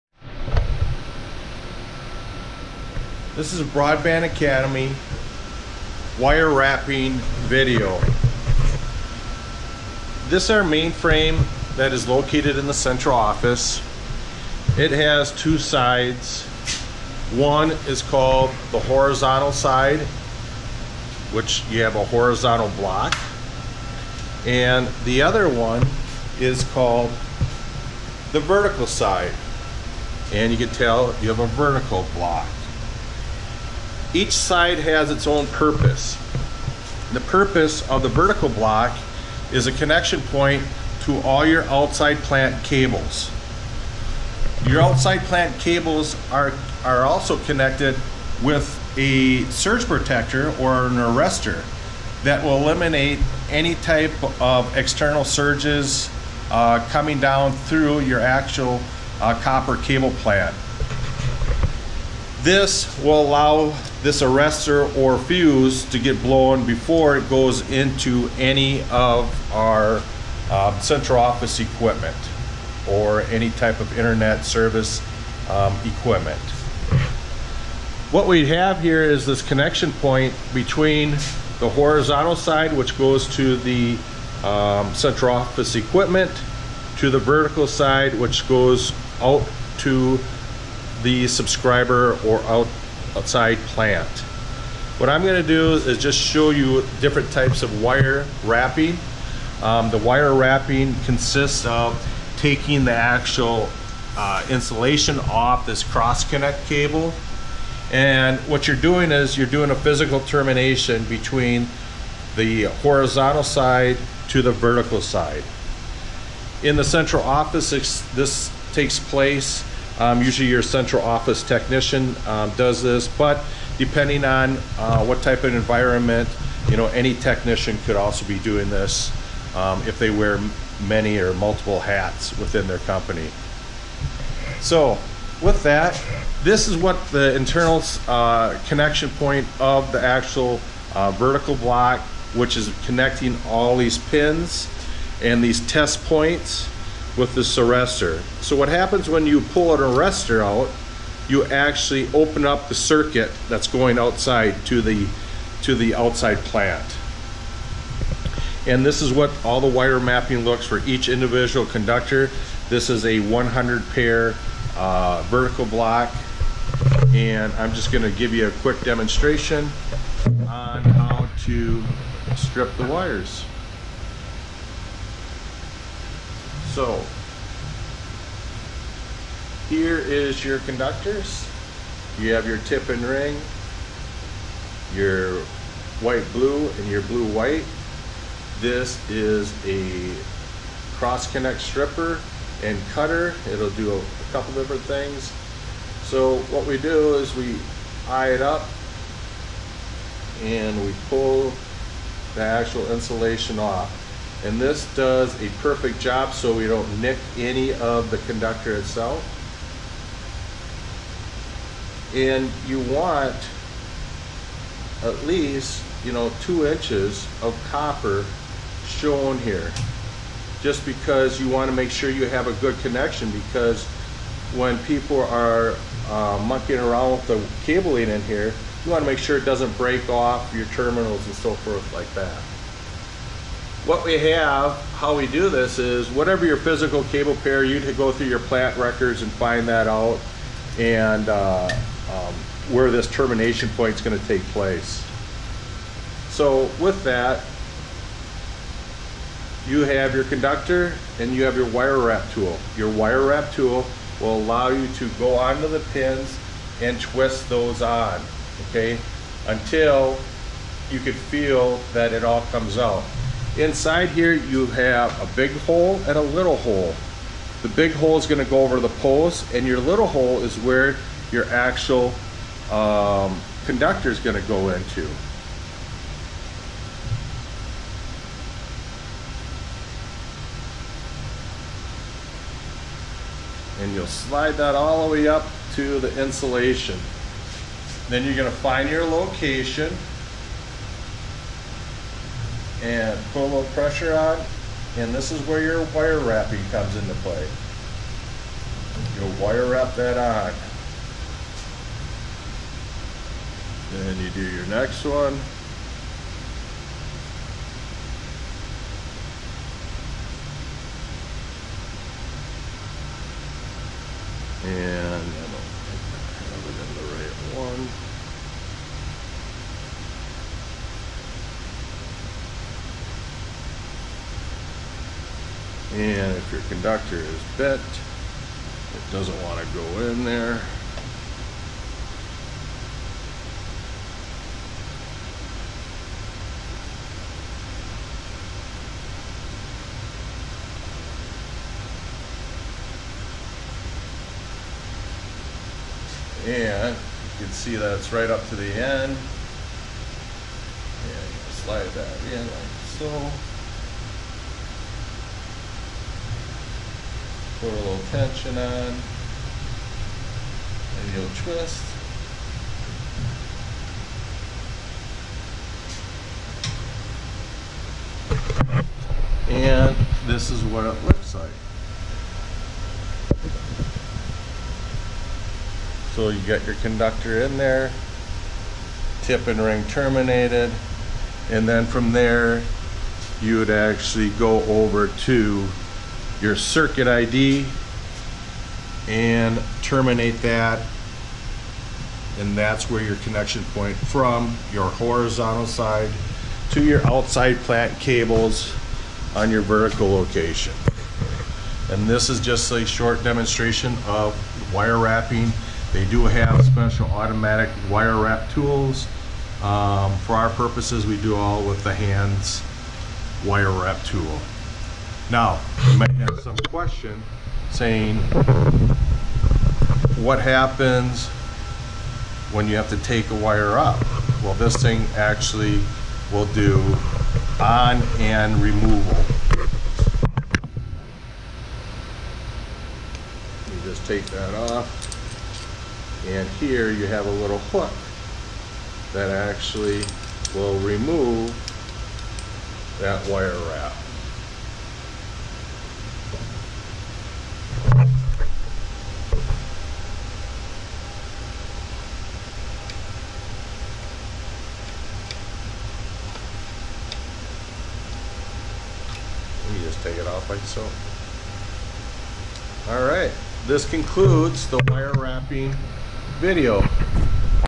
WITC BA Mainframe Wirewrapping
This video explains wirewrapping on the mainframe.